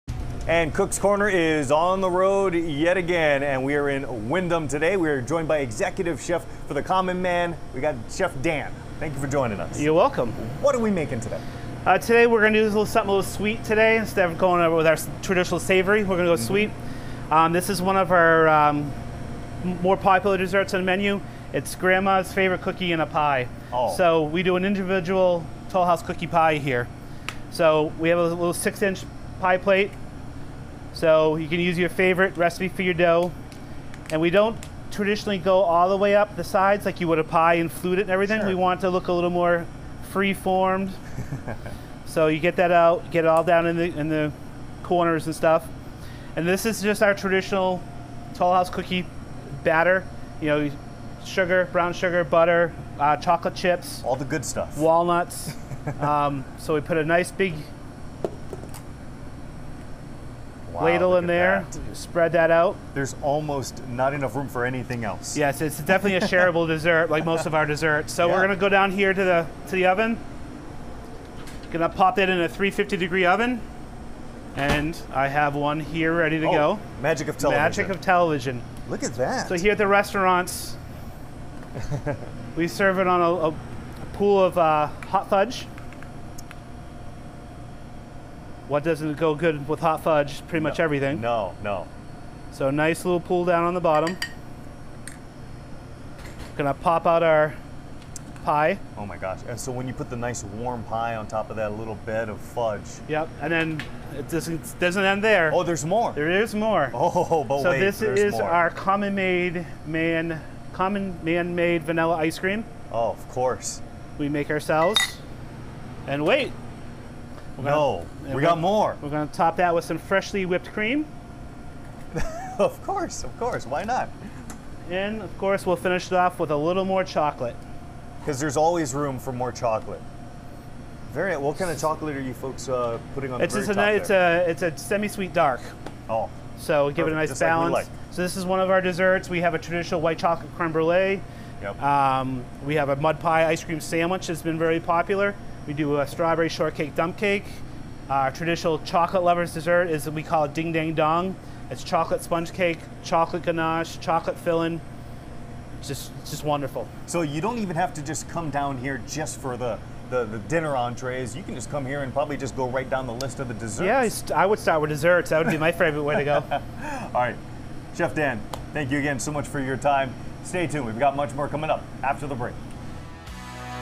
Cook's Corner: Cookie in a pie
Executive Common Man chef Dan St. Jean shows how to prepare cookie in a pie.